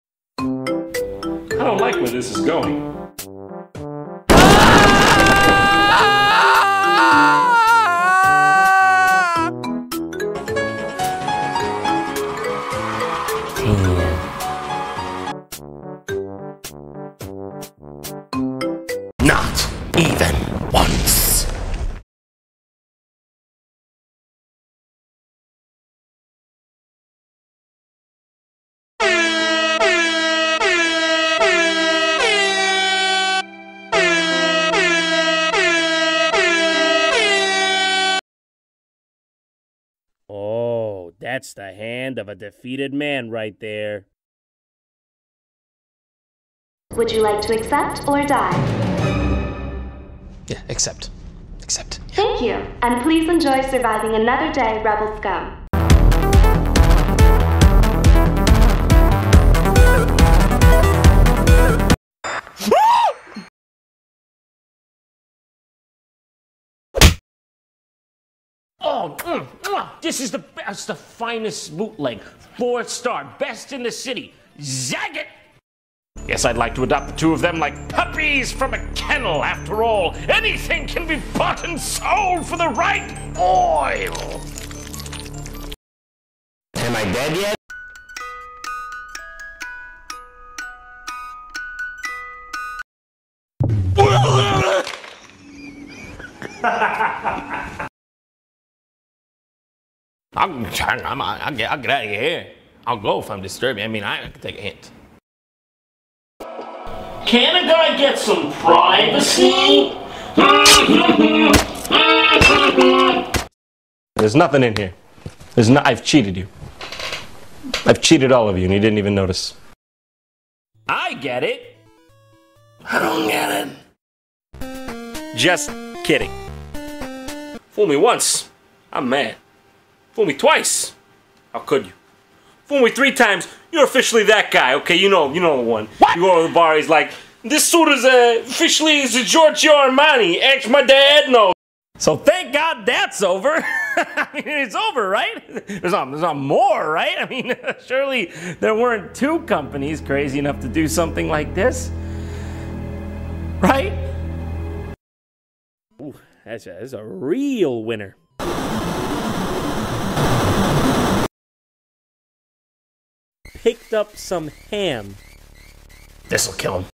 (TBB) [SFM] Waat! pt 5
i completly dont know waat happened to the lip sinc WELP. i cant belive i made this one well ENJOY XD yes yes ik i said the 4th one was the final one but screw that! im gunna make this alot more times but this time with other voices also if you want to make reactment videos to this go ahead X3 /Subscribe for more Everybunny.